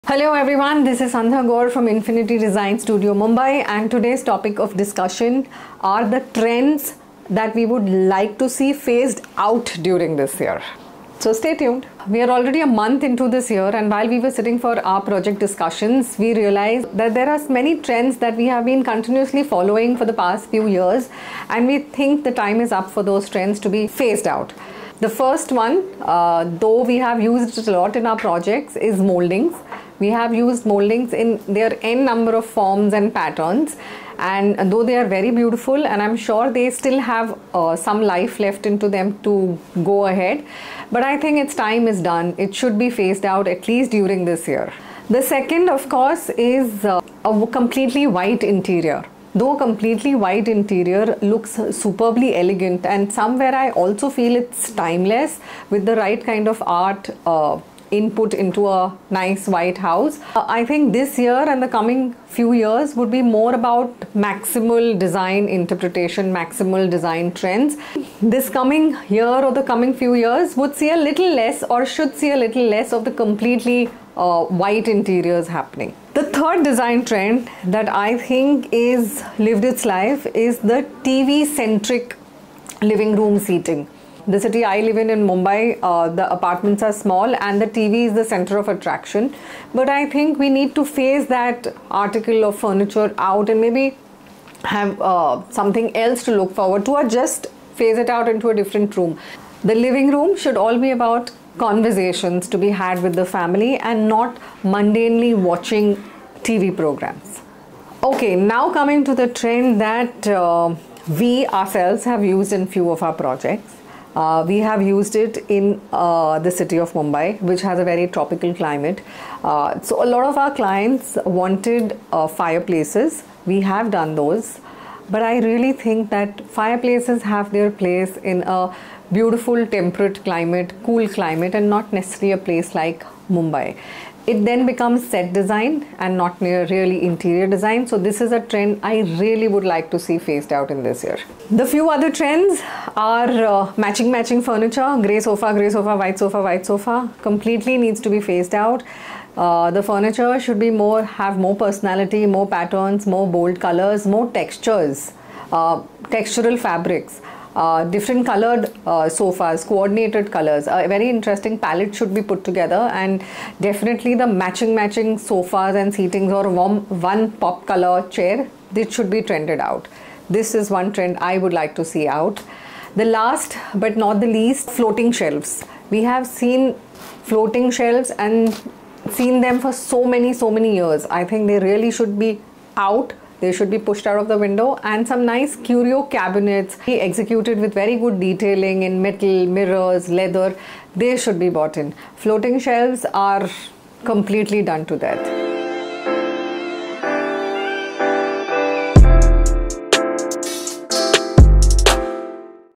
Design Trends that I am "quiet quitting" in 2023 | Infinitti Design Studio
There is a lot to be said for 'quiet quitting' – the affirmative life trend that is making us all reevaluate our tolerances. It's about giving voice to disengaging with anything that no longer makes us happy, and while this 'trend' is reflective of pandemic-induced burnout at work, it has become something that we can apply to other areas of our lives too, not just our careers. BY JENNIFER EBERT